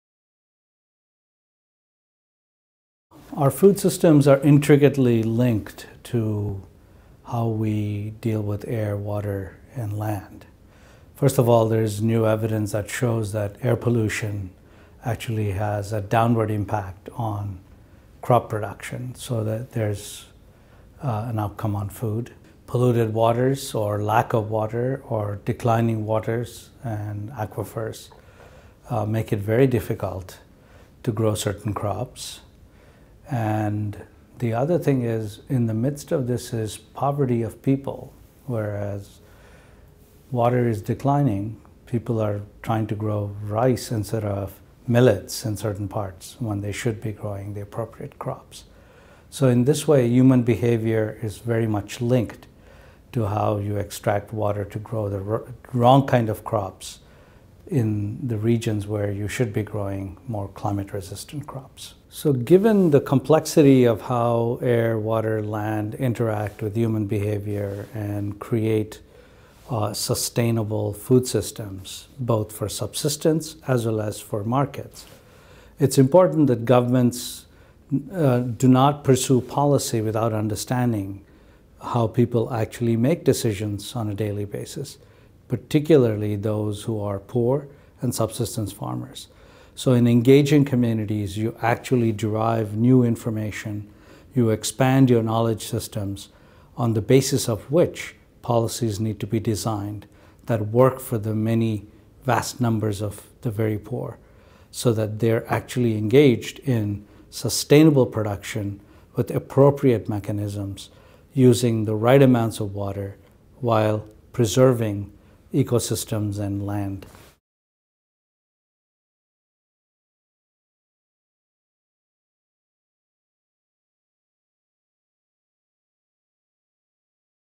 Gautam N. Yadama: Engaging Communities to sustain social, ecological and livelihood Systems
"It’s important that governments do not pursue policy without understanding how people actually make decisions on a daily basis, particularly those who are poor and subsistence farmers"

Gautam N. Yadama, Dean and Professor, Boston College School of Social Work